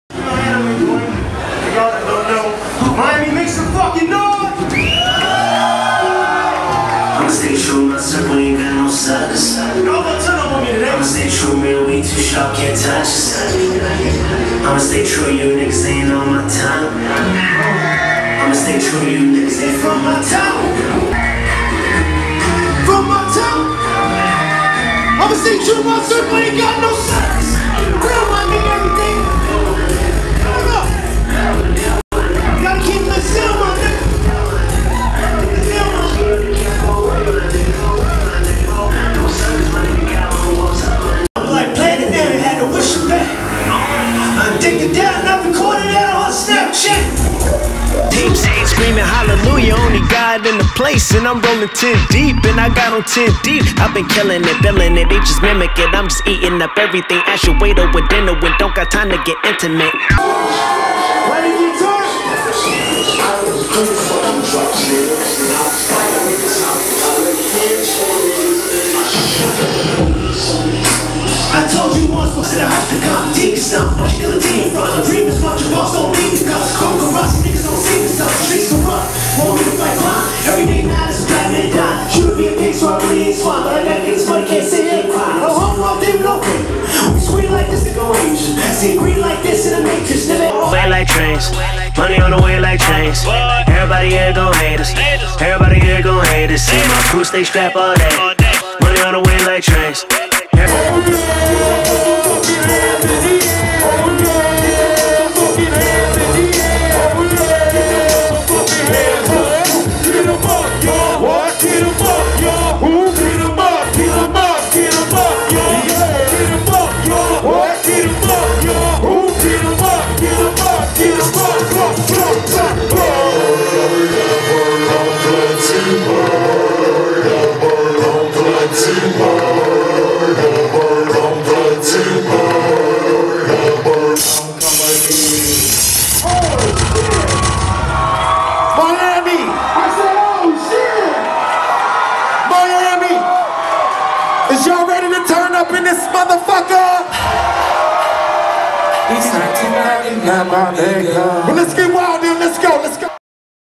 Cashamill Animeboy opens for BONE THUGS-N-HARMONY concert
Cashamill opens for Legendary rap group Bone Thugs-n-Harmony which drawed a much older crowd and still made the old heads jam out but no mosh pits for OG's lol

FOR ALL FLORIDA ARTIST